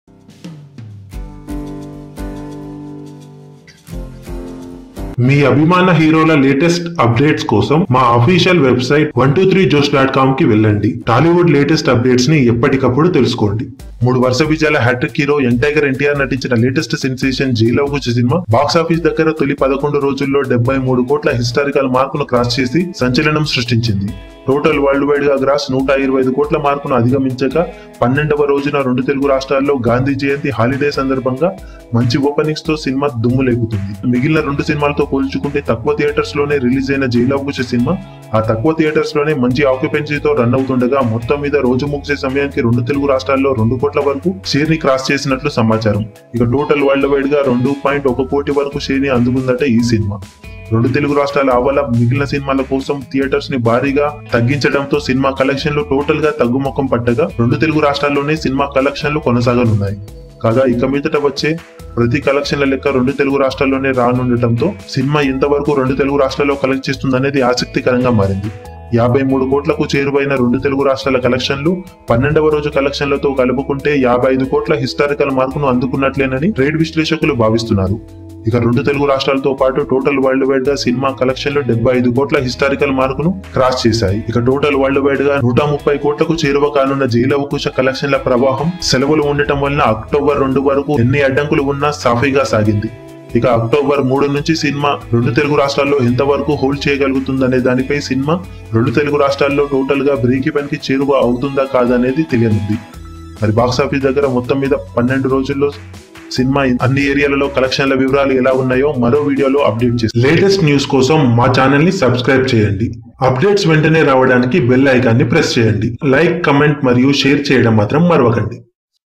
12 రోజుల్లో 130 కోట్లు!!....యంగ్ టైగర్ భీభత్సం సృష్టించాడు || Jai Lava Kusa 12 Days Collections
12 రోజుల్లో 130 కోట్లు!!. యంగ్ టైగర్ భీభత్సం సృష్టించాడు || Jai Lava Kusa 12 Days Collections